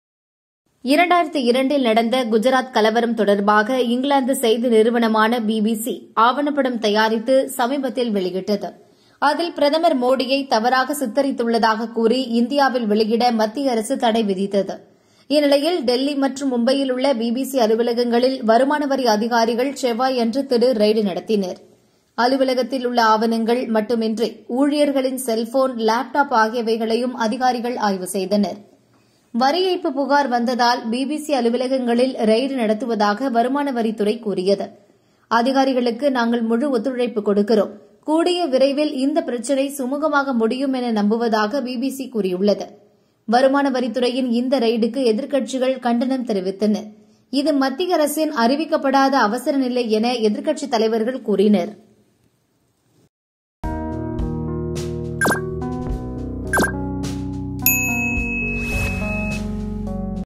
பிபிசி ஊழியர்களின் போன், லேப்டாப்பை தோண்டி துருவும் ஐடி அதிகாரிகள்
பிபிசி ஊழியர்களின்
போன், லேப்டாப்பை
தோண்டி துருவும்
ஐடி அதிகாரிகள்